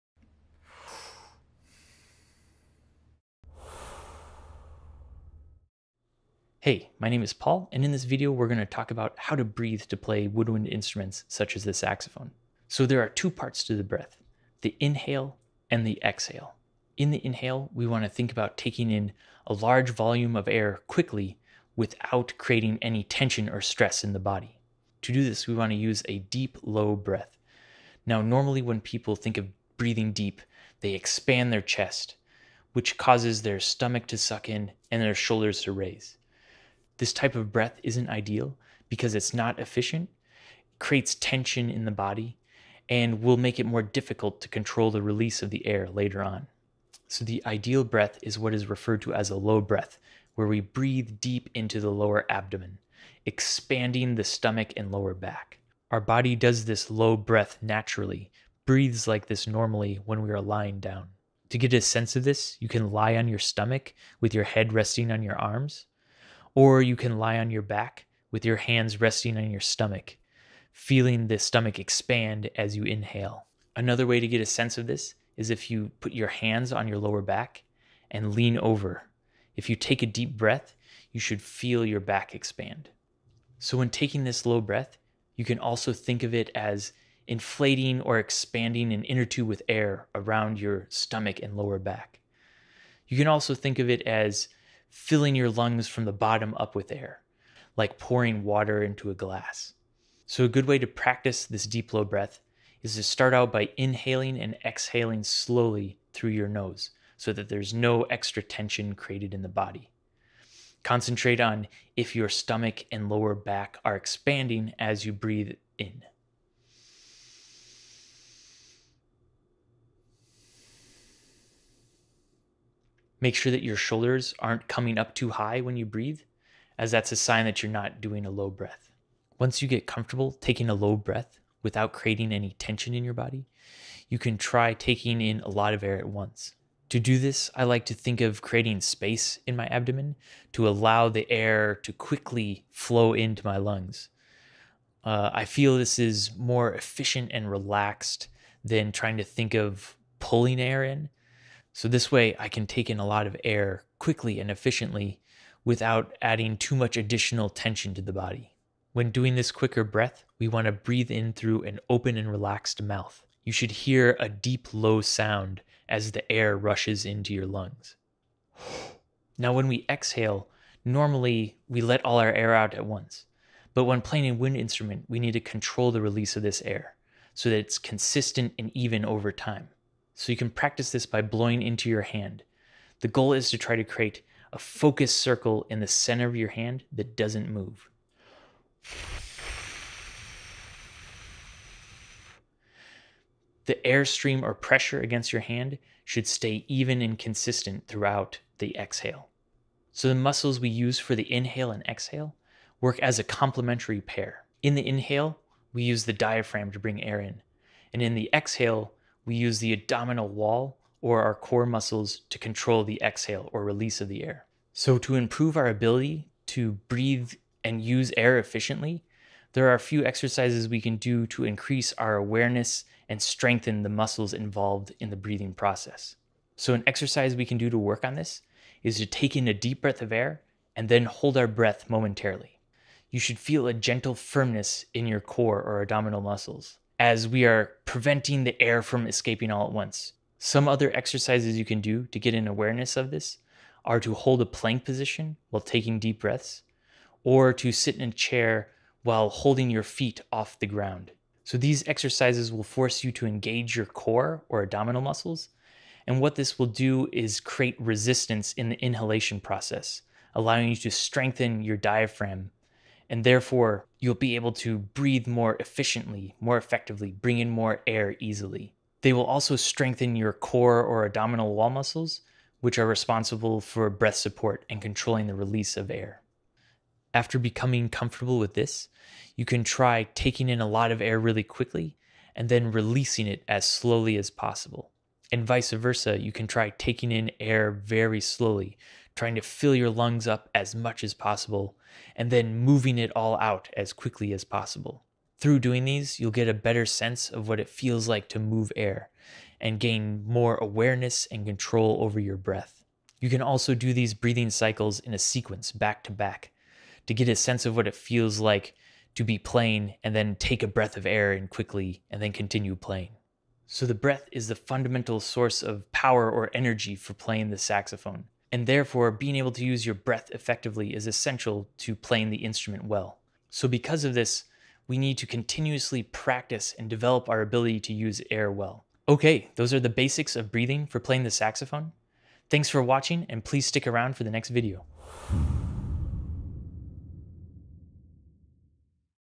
Saxophone Basics // Breathing for Playing the Saxophone
This tutorial explains the basics of how to breathe for playing a woodwind instrument such as the saxophone. This covers developing the correct breathing technique for inhaling and moving air through the instrument and exercises to strengthen and improve the efficiency of the breath.

Intro 0:00
Technique of Breathing 0:12
Developing the Low Breath 0:55
The Quick Inhale 2:09
The Exhale 2:50
Exercises 3:34
Conclusion 5:13